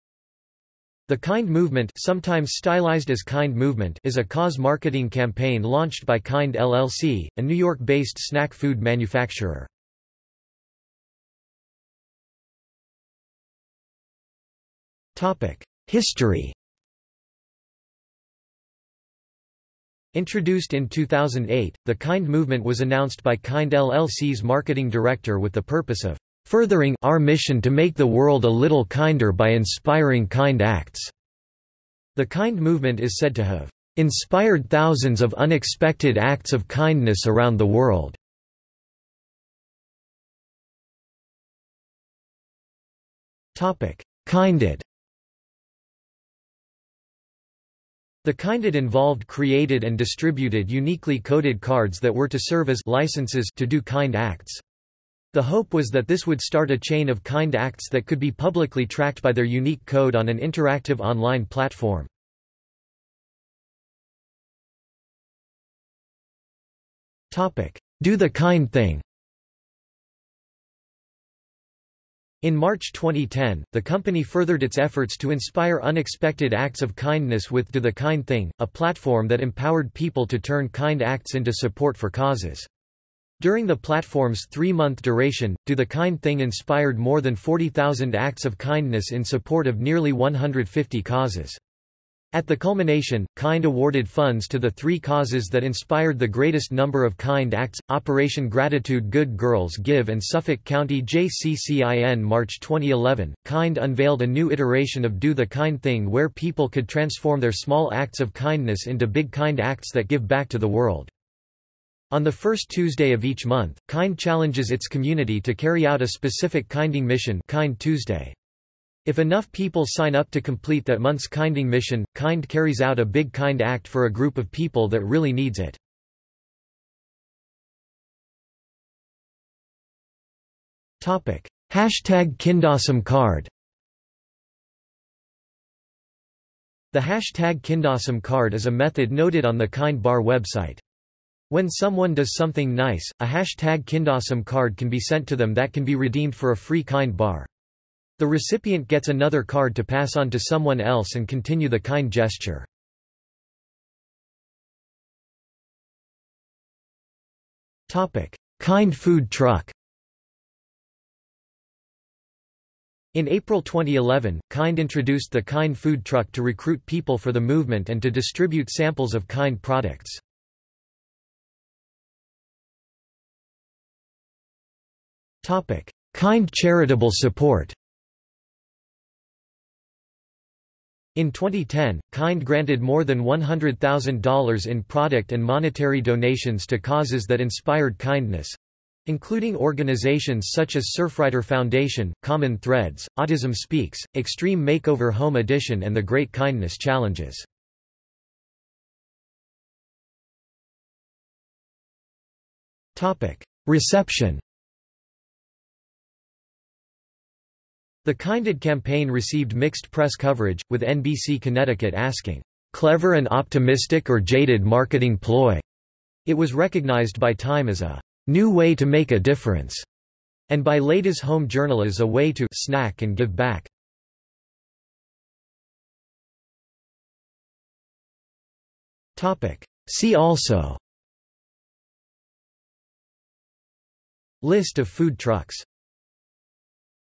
KIND Movement | Wikipedia audio article
This is an audio version of the Wikipedia Article:
KIND Movement

00:00:11 1 History
00:00:40 1.1 iKinded/i
00:01:04 1.2 iDo the KIND Thing/i
00:02:07 1.3 Kindawesome Card
00:02:34 1.4 Kind Food Truck
00:02:52 1.5 Kind Charitable Support
00:03:19 2 Reception
00:03:45 3 See also

- Socrates

SUMMARY
The Kind Movement (sometimes stylized as KIND Movement) is a cause-marketing campaign launched by Kind LLC, a New-York-based snack-food manufacturer.